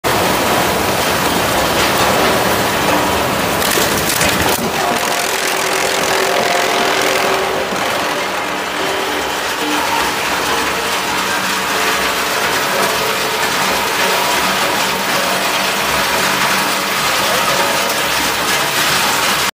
Waste Wood Drum Chipper Machine Wood Chipper Forestry Machinery
Advantages of drum chipper

1. Powerful: can process various materials efficiently.

2. Long service life: the machine's knife roller, knife plate, and blade are made of high-quality materials.

3. The chipper has an independent electric control cabinet, which is safe to use and easy to operate.
Company address: Nautical Road East, Zhengzhou Economic Development Zone, Henan, China